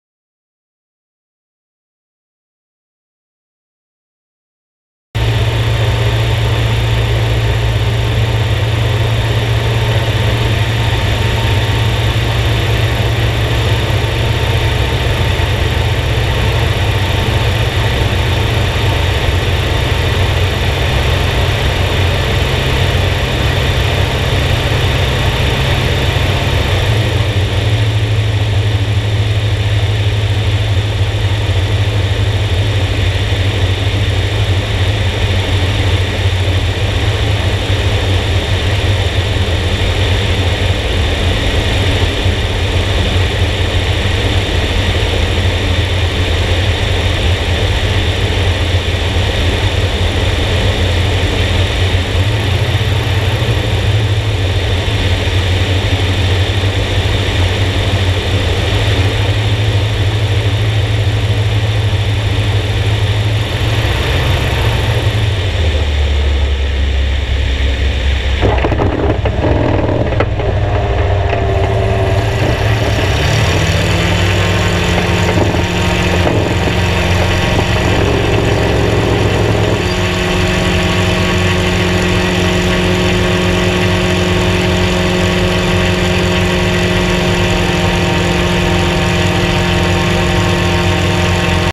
Ultralight Aviation. A Short Field landing with a Wheelie.
Learning from the Pro's.  Rogelio Corvo instructing on a Drifter.